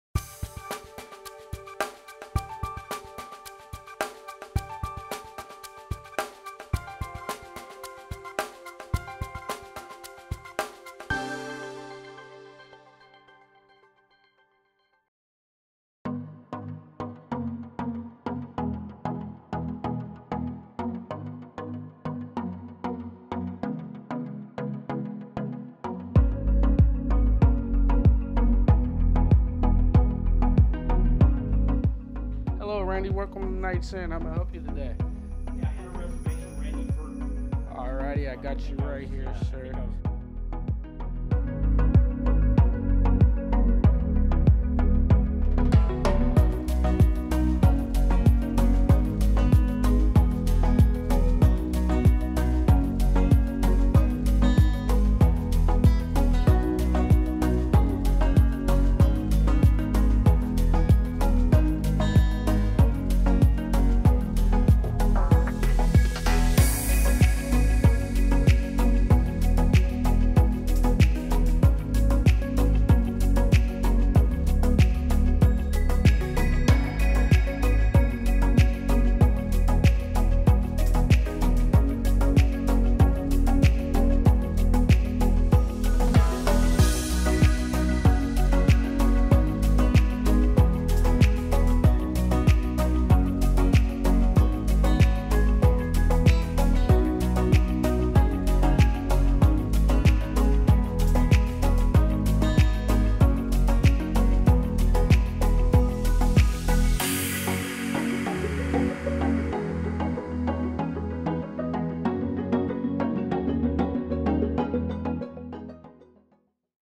Welcome to Knights Inn - Jacksonville, Illinois
A short tour of the Knights Inn Motel located in Jacksonville, Illinois

Tour provided by Jacksonville Area Convention and Visitors Bureau and Steve Weber Films